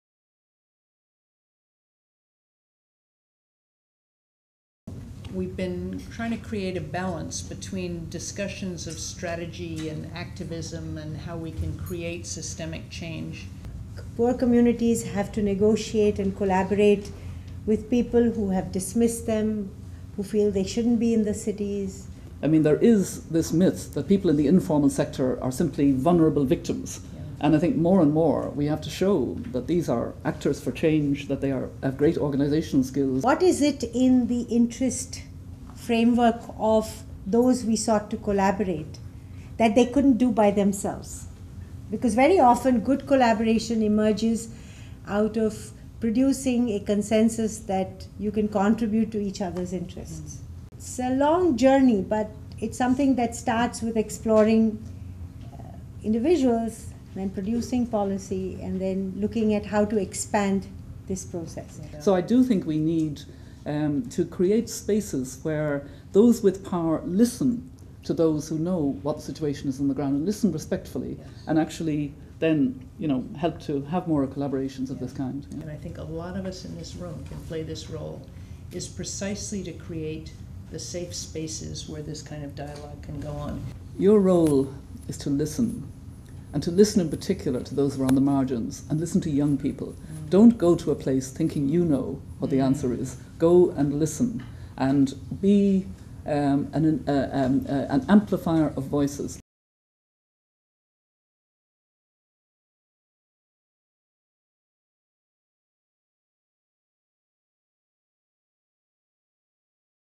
Leading Collaboration for Social Change
Discussion at the Synergos 2009 Global Philanthropists Circle Annual Meeting with Sheela Patel and Mary Robinson, facilitated by Peggy Dulany.